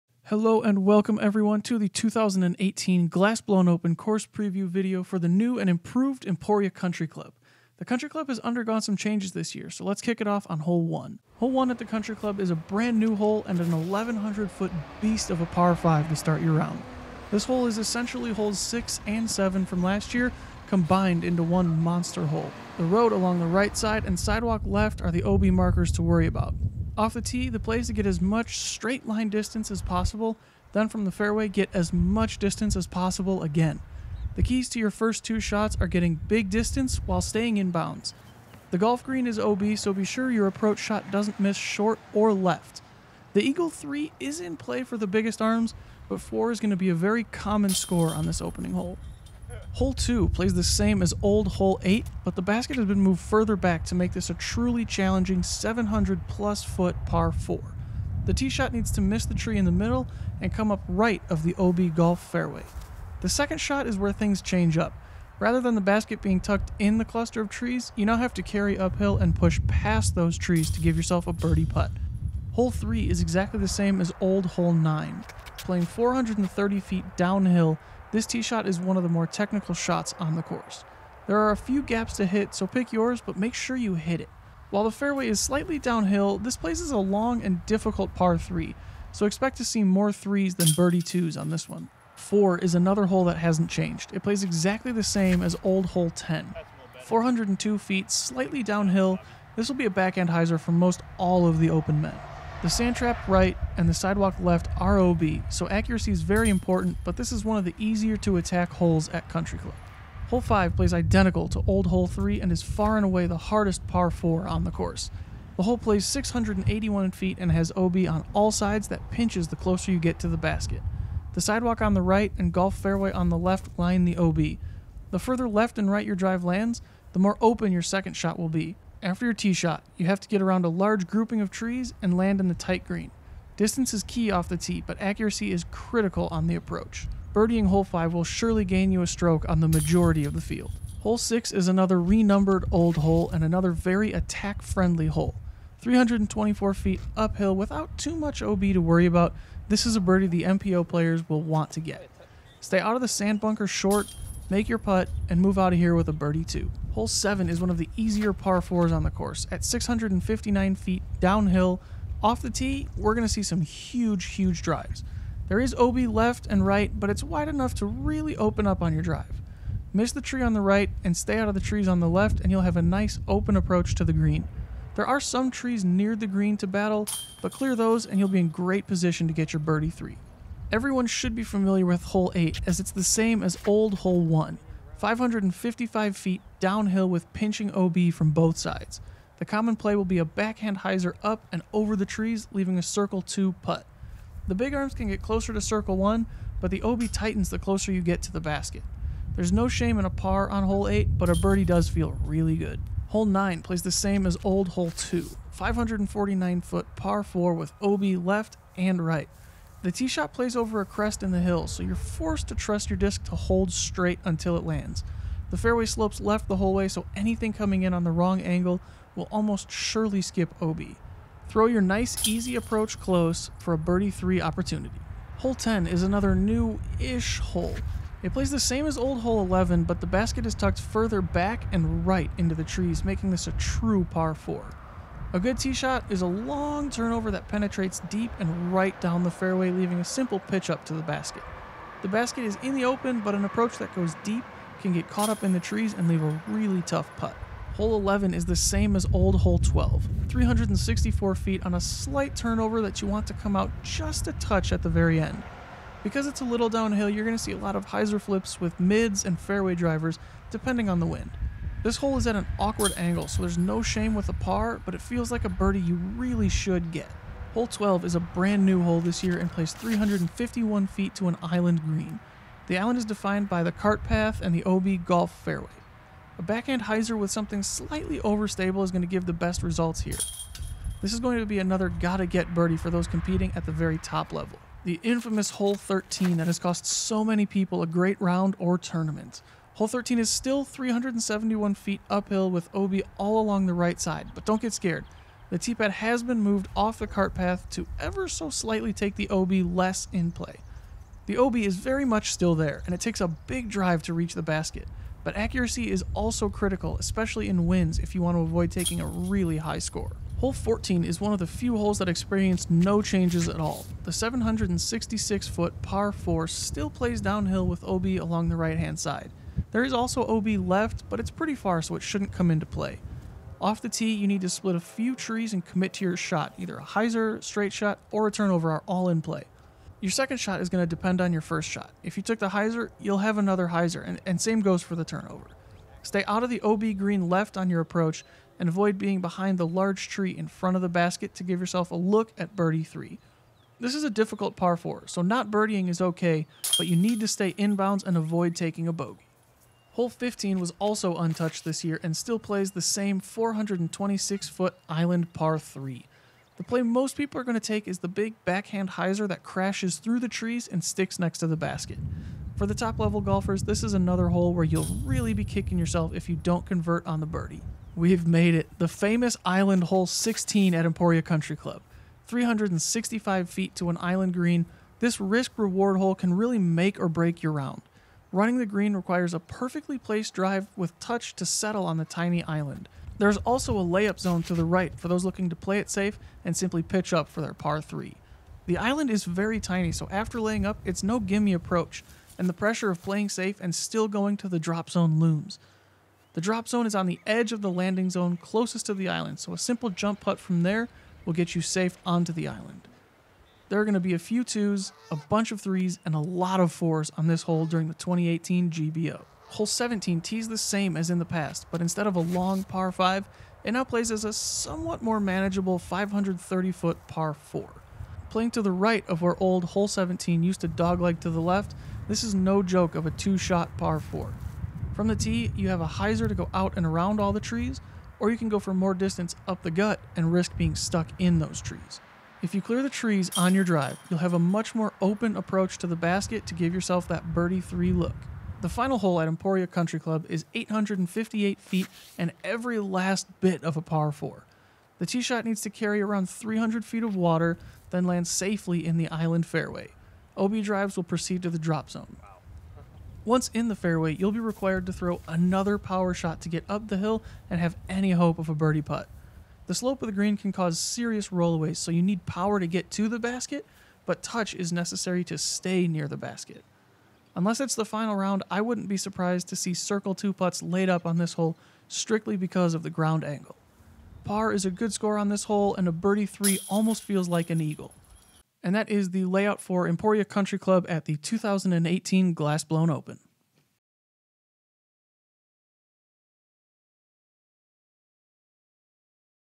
2018 GBO Course Preview: Emporia Country Club
Eric McCabe and Zach Newhouse walk us through the course changes to the 2018 Glass Blown Open layout for the Emporia Country Club course.